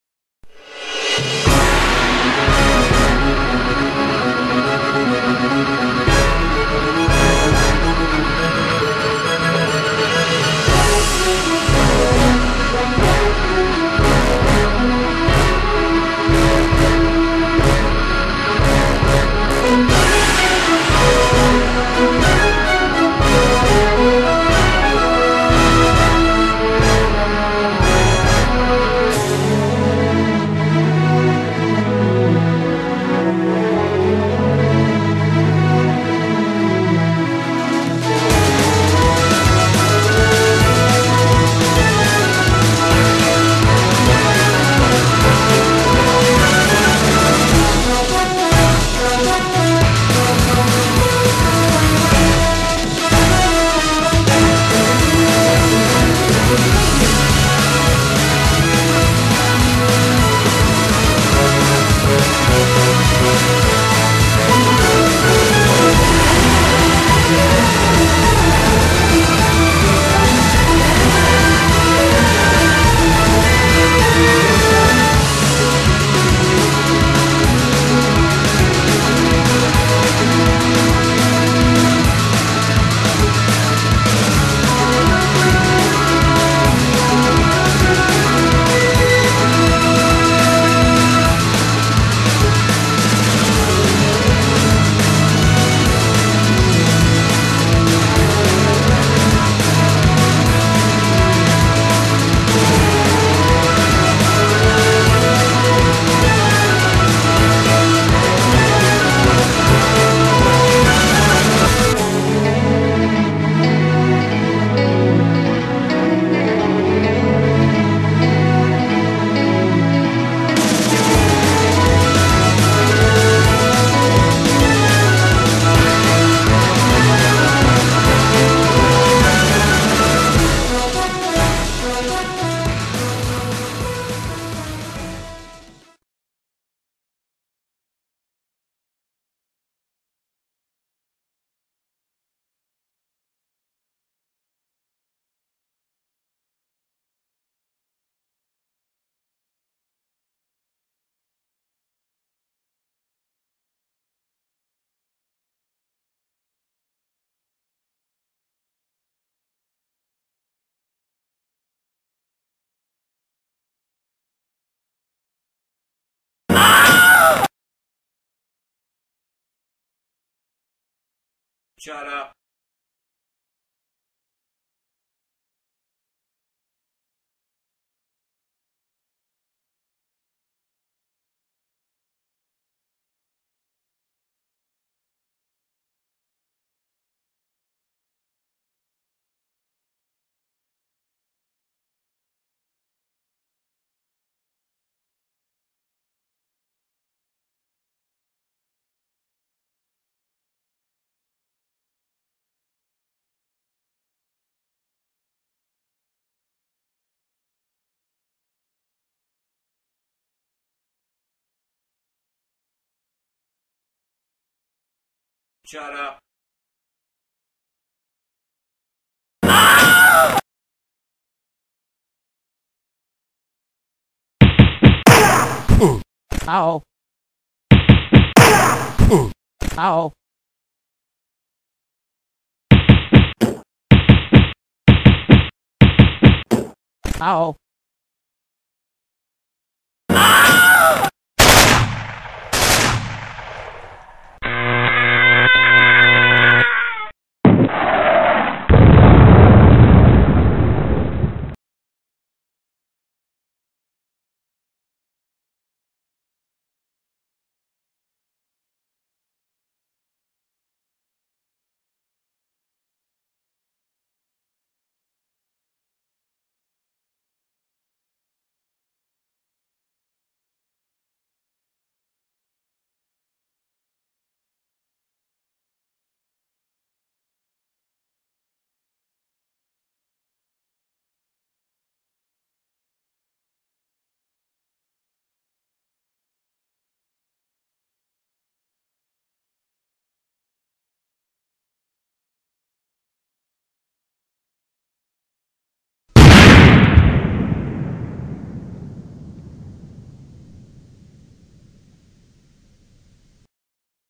The Beginning....wmv
This is the beginning of the series. I'm sorry it came out late, but I was caught up in the first two and the theme song. Sprites and Music not by me.